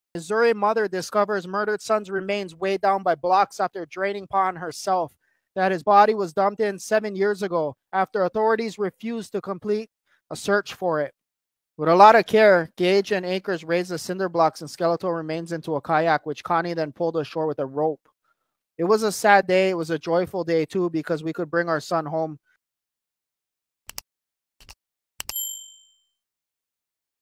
Missouri Mom Drains Pond Herself To Recover Murdered Son’s Remains Connie Goodwin
Link to video A Missouri woman recovered the rest of her murdered son’s remains after draining a pond where his partial remains were found in 2017, two years after he went missing.

Connie Goodwin and her family used a rented sump pump to drain the Poplar Bluff lake-turned-pond — after it was initially drained to recover 40% of the 32-year-old’s remains in 2017 — last Saturday, People reports.

It took them two hours to find her son Edward Goodwin’s bones.

“I told [the Butler County Sheriff’s Department] that if they don’t [drain the remaining water], I told them that one of these days, I’m going to do it myself,” Goodwin, who ultimately undertook the effort with Edward’s son Gage, 22, and her husband Ed, told the outlet. “We saw two bones sticking up, so we kept pumping and then the next thing you know we started seeing the concrete blocks and then barbed wire and more bones.”

The concrete and wire were used to send Edward Goodwin’s body to the depths of that former lake.

The remains were confirmed to belong to the late Goodwin after his mother phoned Butler County Coroner Jim Akers, who fished out the rest of the discovery by hand, retrieving it from what he told People was “very thick” mud, “infested with wildlife.”

The fate of Edward Goodwin, who disappeared in 2015, was unknown until his partial remains were found two years later. Former friends Rickey Hurt and Eldrid Smith were arrested in 2017 and pleaded guilty last year to killing Goodwin.

Goodwin’s mother remembers him as “a loving father” and " a loving son” who would “do anything for anyone.

“He loved his whole family and his family loved him,” she said